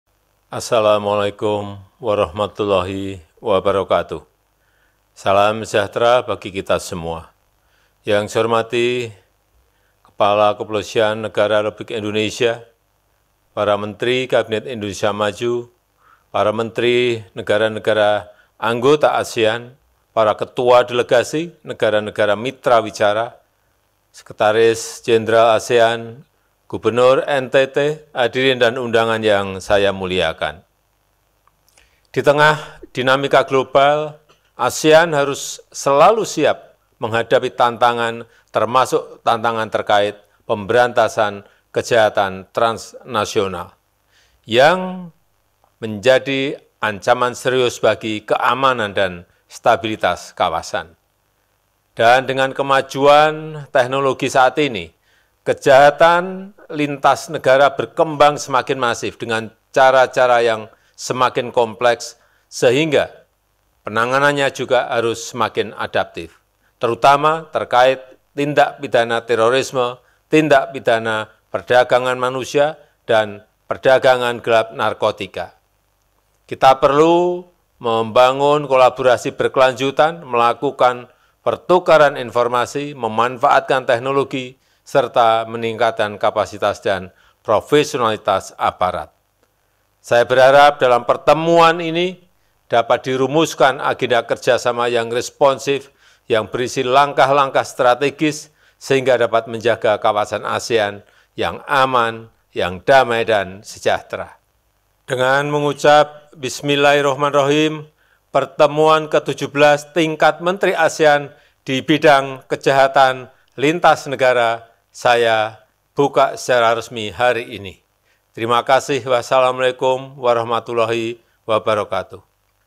[FULL] Pidato Presiden Jokowi Buka ASEAN AMMTC ke-17, Bahas Kejahatan Transaksional hingga Teroris
Tribunpapuabarat.com - Presiden Jokowi membuka ASEAN Ministerial Meeting on Transnational Crime (AMMTC) ke-17 di Labuan Bajo, Manggarai Barat, Nusa Tenggara Timur (NTT), Senin (21/08/23) pagi.

Dalam pidatonya, Presiden Jokowi membahas soal penanganan kejahatan transaksional di negara ASEAN.

Termasuk membahas penanganan terorisme di ASEAN.

(Tribun-Video.com/RS)